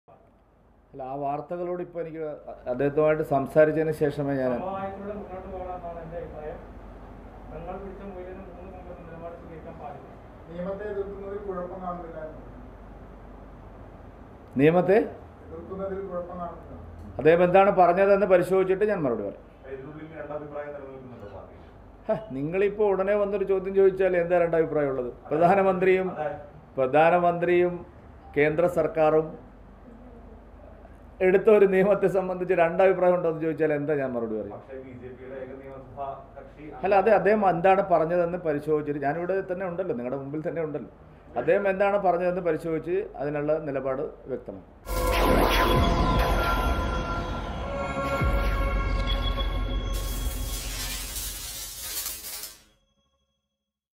ഒ രാജഗോപാല്‍ പറഞ്ഞതെന്തെന്ന് പരിശോധിച്ച ശേഷം നിലപാട് വ്യക്തമാക്കുമെന്ന് കെ സുരേന്ദ്രന്‍
K Surendran On O Rajagopal's support to CM"s resolution